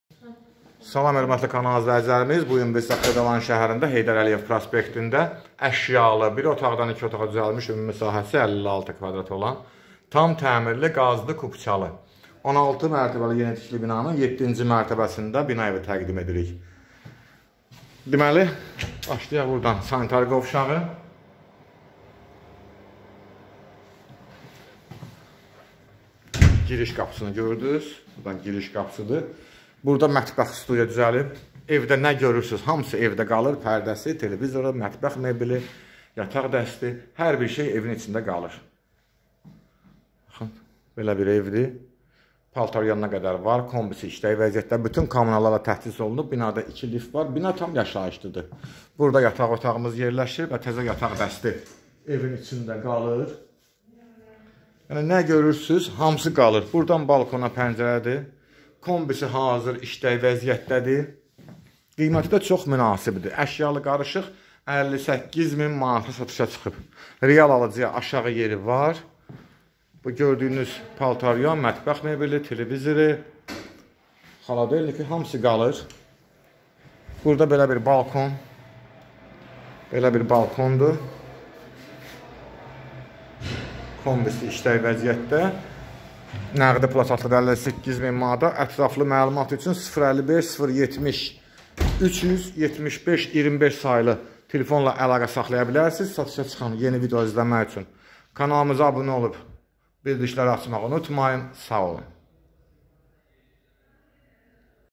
Yeni Tikilidə Təmirli Əşyali Cəmi 58.000 Azn
📍Xırdalan Şəhərində
✅ Binada qaz və digər komunallar mövcuddur. Ev keyfiyyətli materiallar ilə tam təmir olunub və ümumi sahəsi 56 kv m.16/7-ci mərtəbəsində yerləşir.Dayanacaga yaxındır. Təcili satılır.nəğd satışdır.Bina tam yaşayışlıdır. Sənəd kupçadır. Əşyalidi